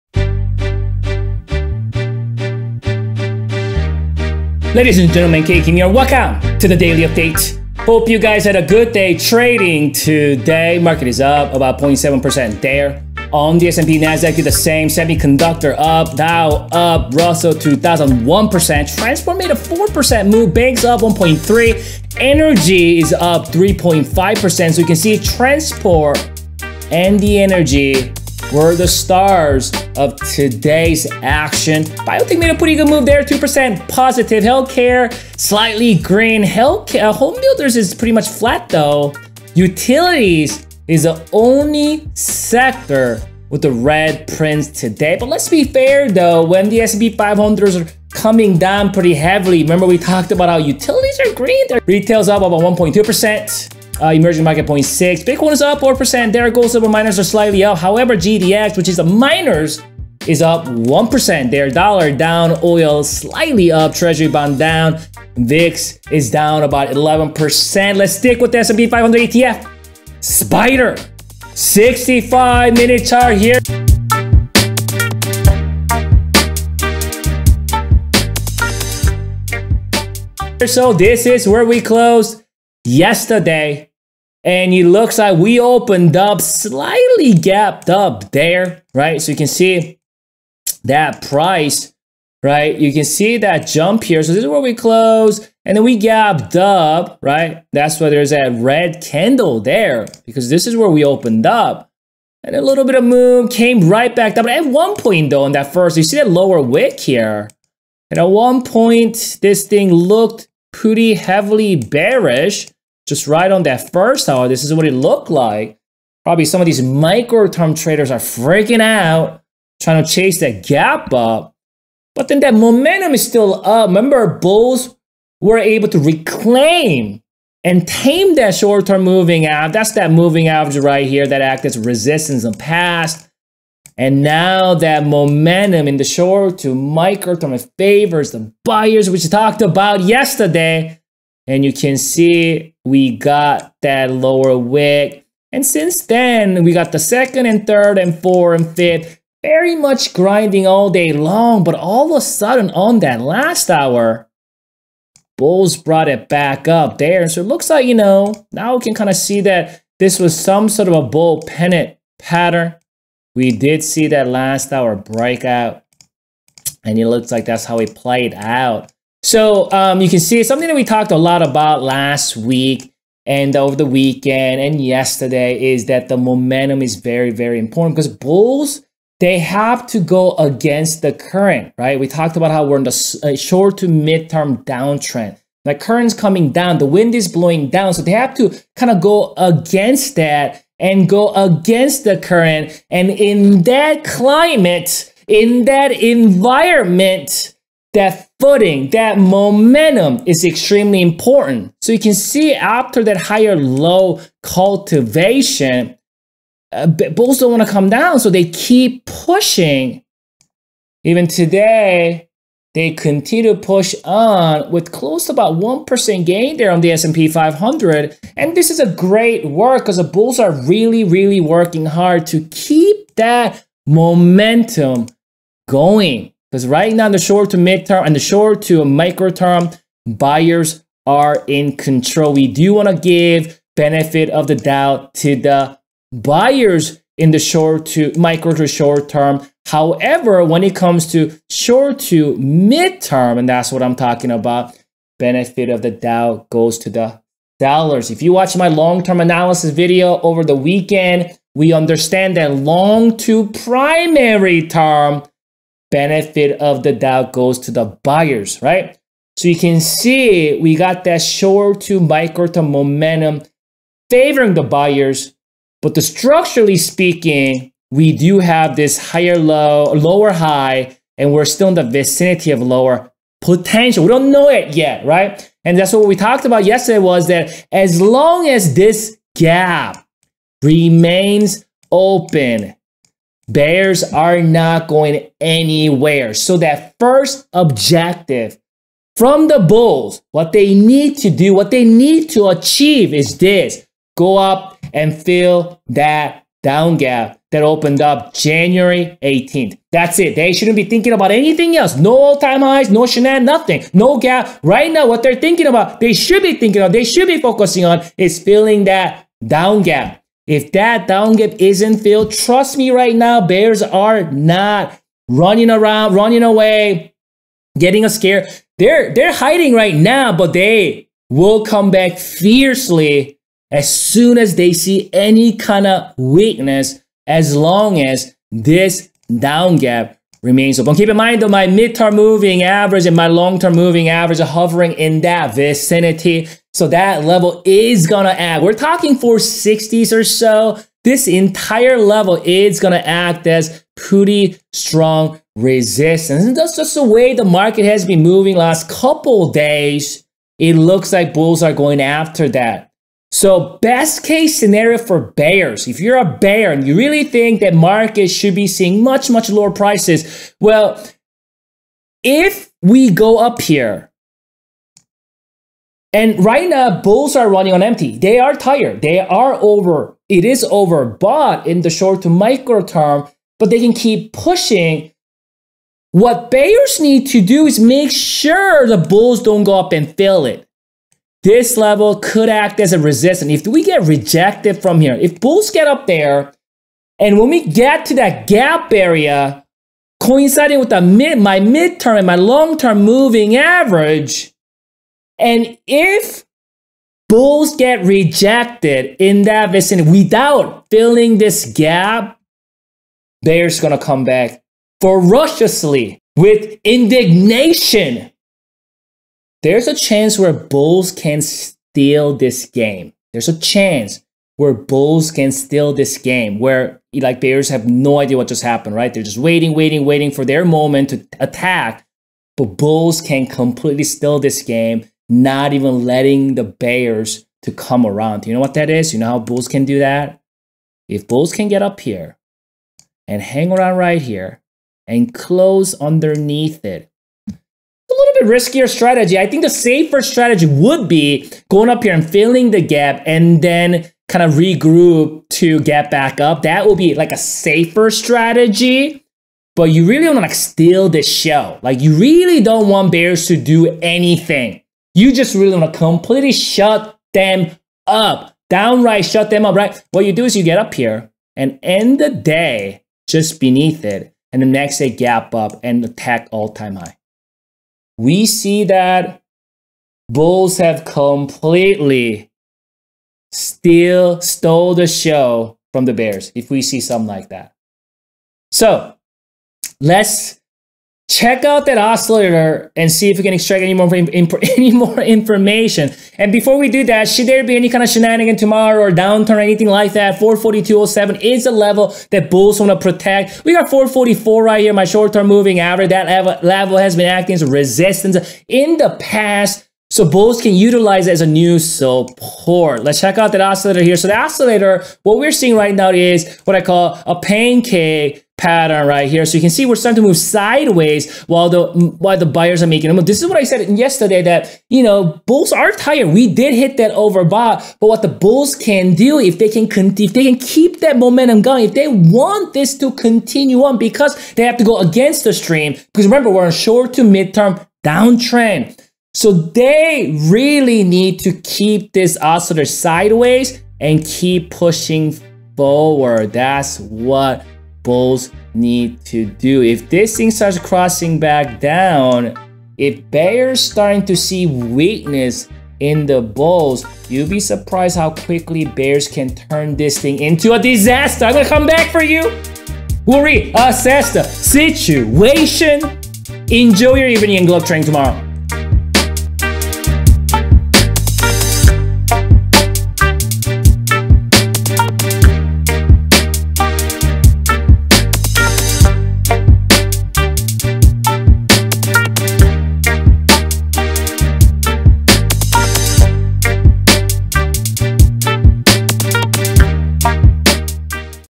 S&P 500: Bulls' Ultimate Plan Exposed?
S&P 500 ETF (SPY) Market Analysis (65min Chart): Market sees another green day today. Isn't the market overbought now? Are the bears dead or are they coming back soon?

Come trade with Kay and see what Kay is currently buying and holding!